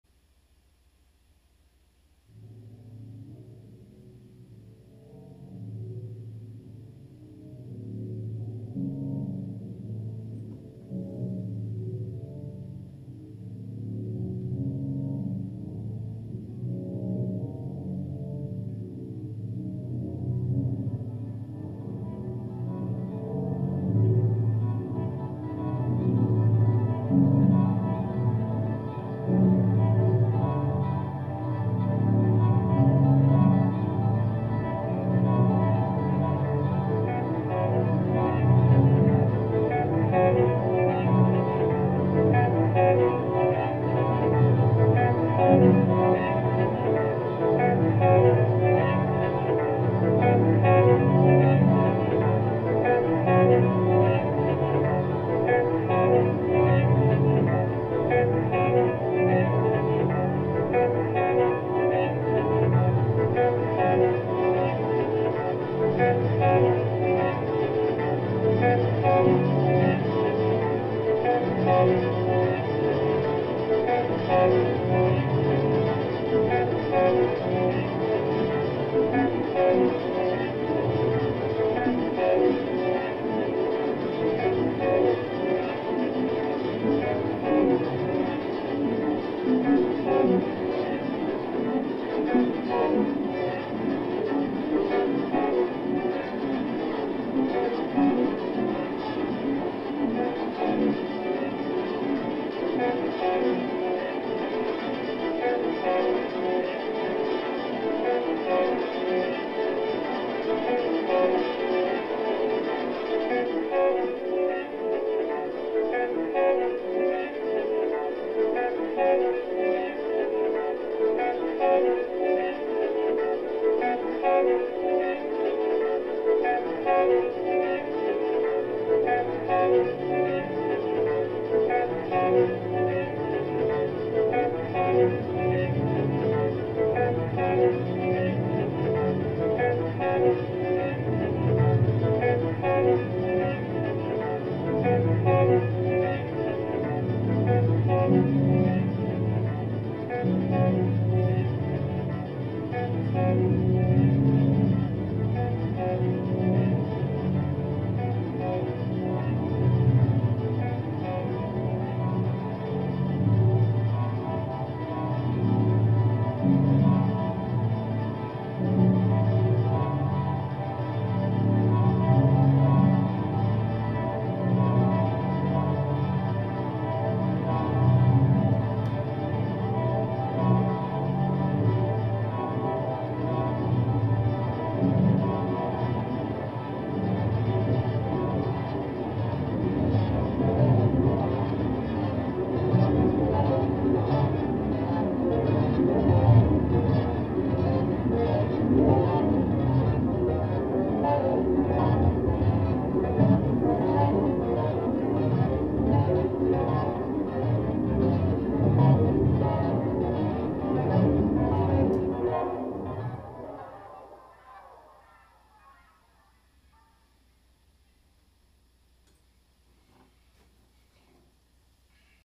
Hob Knobbing Twiddler
Perhaps there is nothing more boring than watching someone twiddle a bunch of knobs...glad to accommodate!  Still, I had a loop left over from an earlier improvisation and decided to focus on it rather than waffle all over the thing.  Seems twiddling won over waffling!